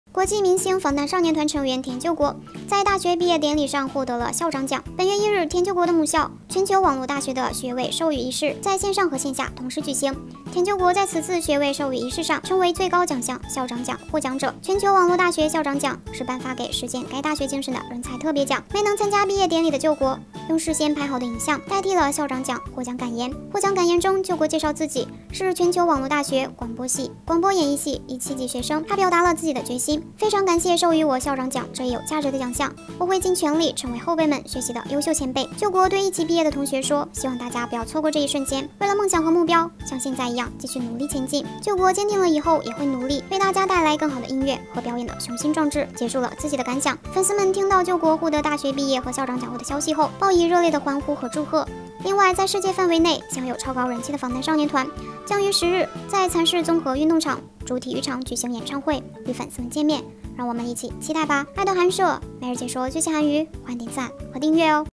BTS 田柾國完成學士學業⋯獲最高獎項「校長獎」光榮畢業網祝賀！
BTS 田柾國完成學士學業⋯獲最高獎項「校長獎」光榮畢業網祝賀！BTS# 田柾國